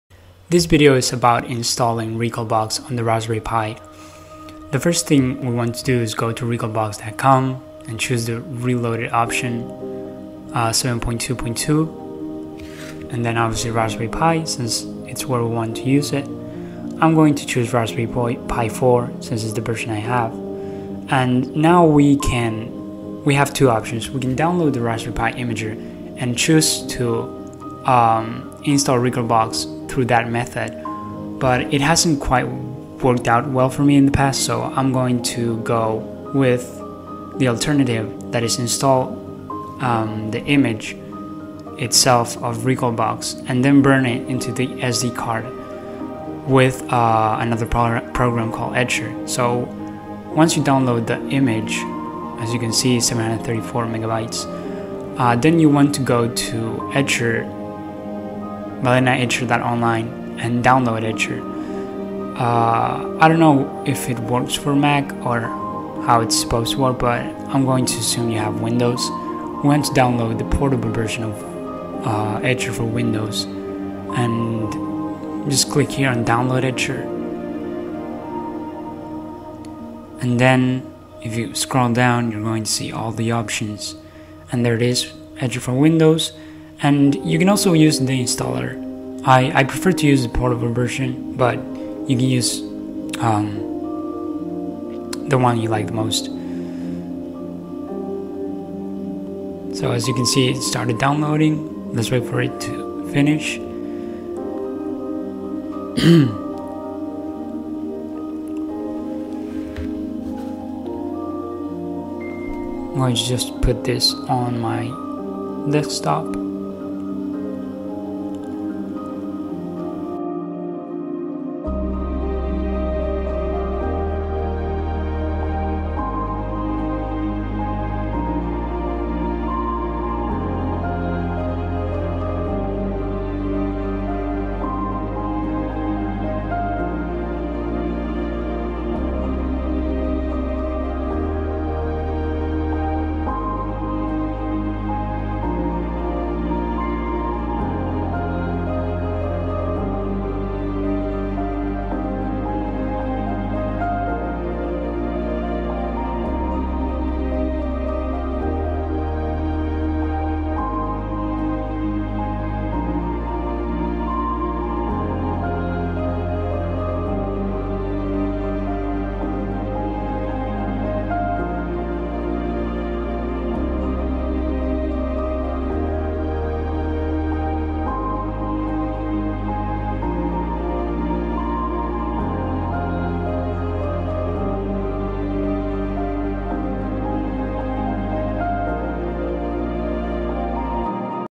How To Play Retro Games On The Raspberry Pi (Recalbox)
This video is about installing Recalbox on your SD Card so you can play retro games on the Raspberry Pi